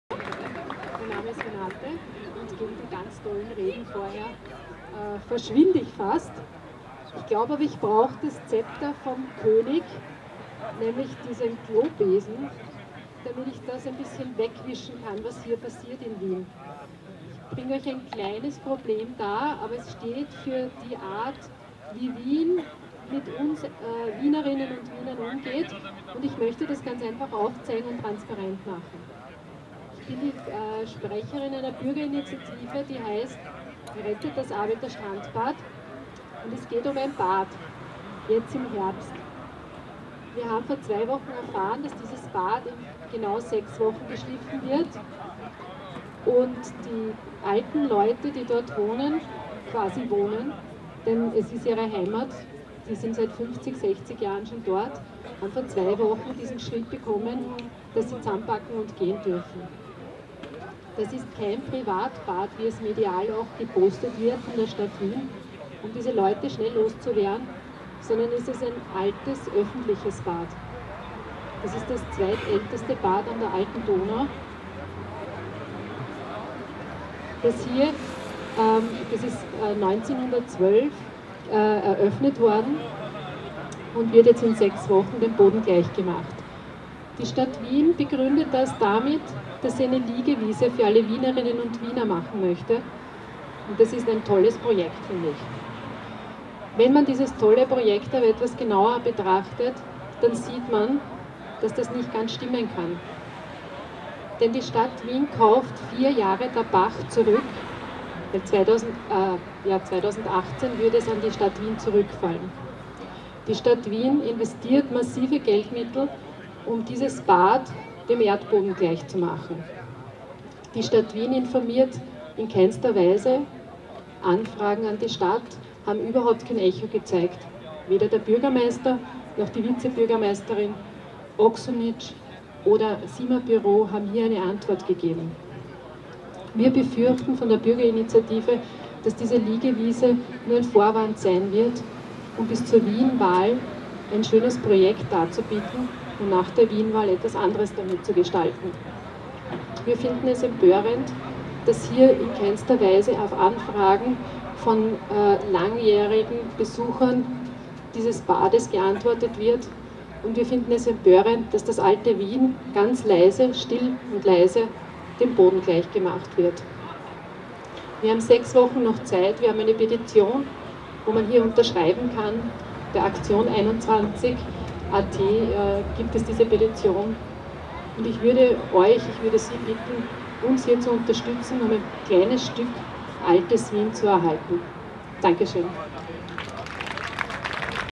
21. Friedensmahnwache in Wien: Bürgerinitiative Arbeiterbad (15.9.2014)
Am 15. September 2014 fand die einundzwanzigste Friedensmahnwache in Wien statt. Wir demonstrieren friedlich für den Frieden in Europa und auf der Welt.